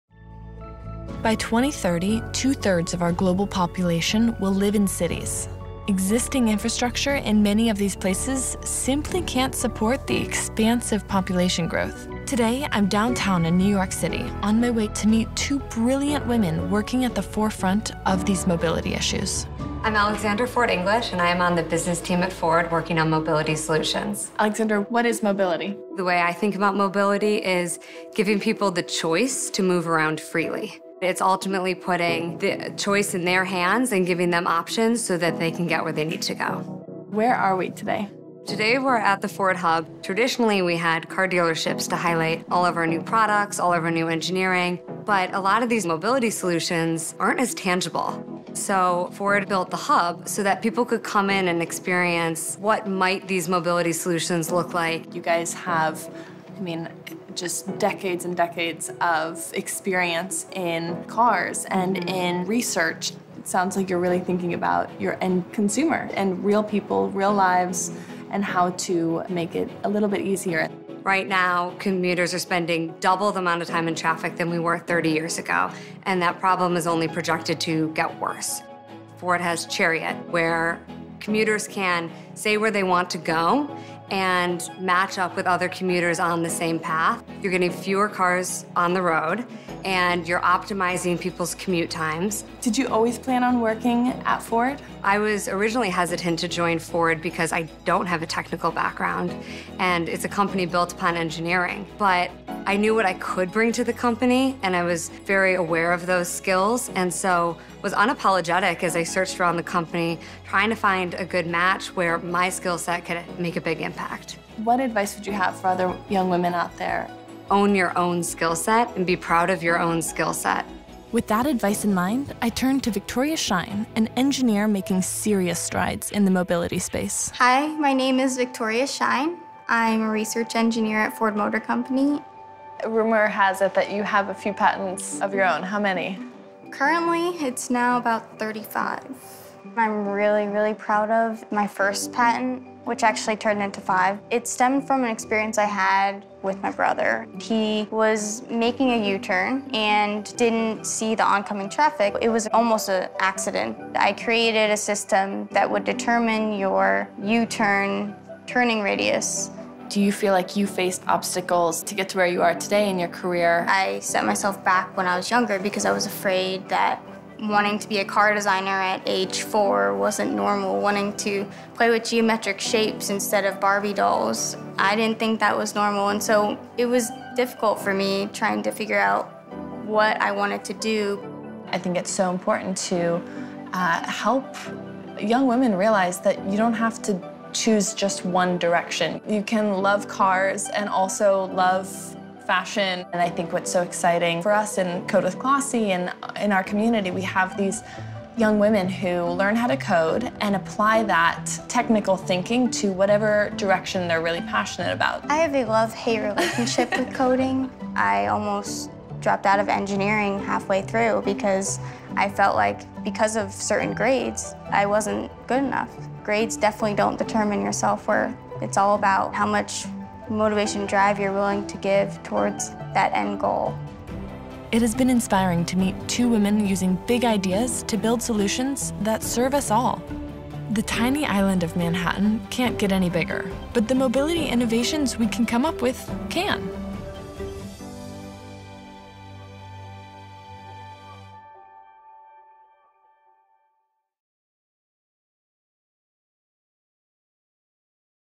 Mobility | Trailblazers of STEAM | Karlie Kloss
In collaboration with Ford STEAM Experience, I set off on a mission to learn from a few of the brightest minds shaping what’s next across four distinct industries: mobility, space, food, and gaming.

a Movie by Karlie Kloss & Klossy Productions
Directed by Eliza McNitt

Producer: Annalora von Pentz & Libby de Leon
Editor: Alicia Shepard
Score by Mark Solomich
Producer: Nic Stanich
Cam Op: Olivia Booker
Movi Op: Andi Obraski
AC: Alexandra Bock
Gaffer: Ariel Nehorayoff
Sound: Mielle Ezra
Production Assistants: Marissa Grinstead, Heather Taylor, Kiera Wilson